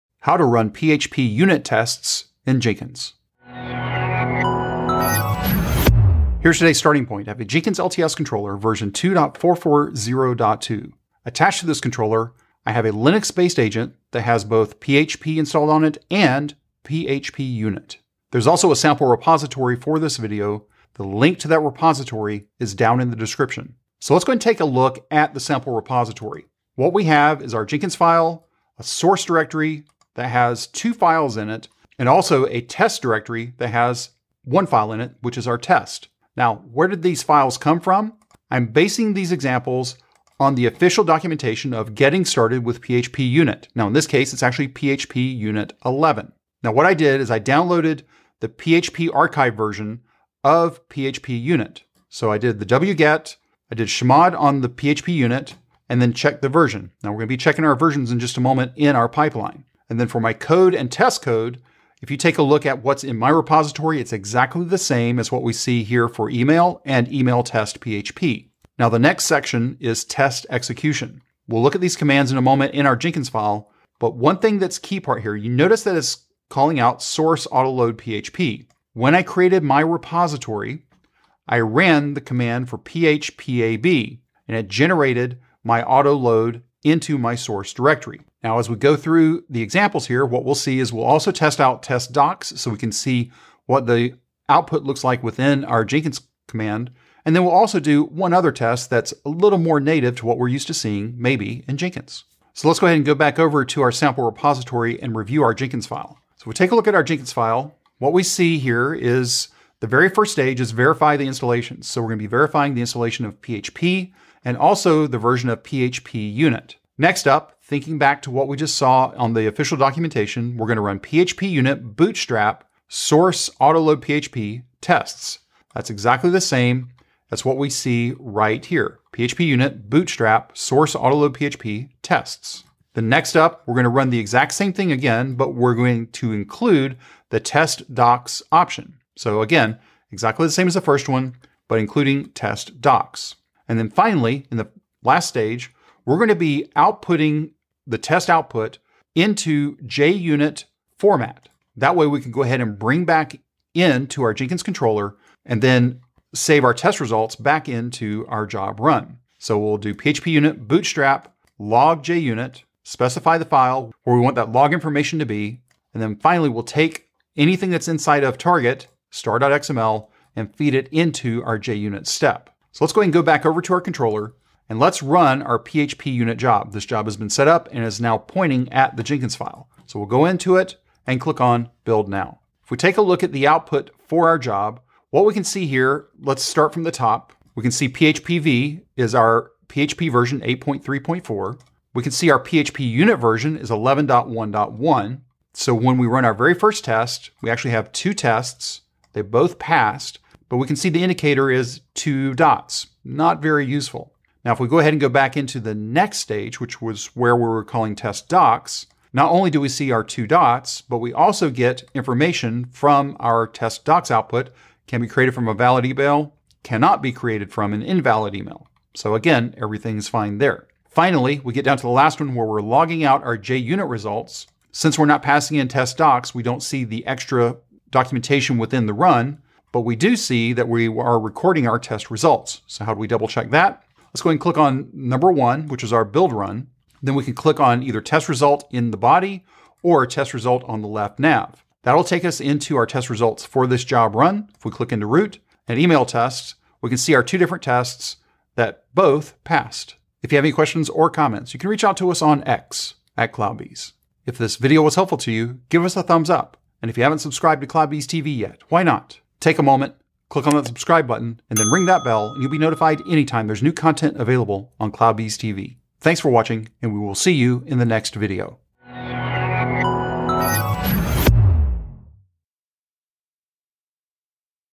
How To Run PHPUnit Tests in Jenkins
Timecodes ⏱:

00:00 Introduction
00:06 Starting point
00:23 Review the sample repository
02:52 Run the job and review the results\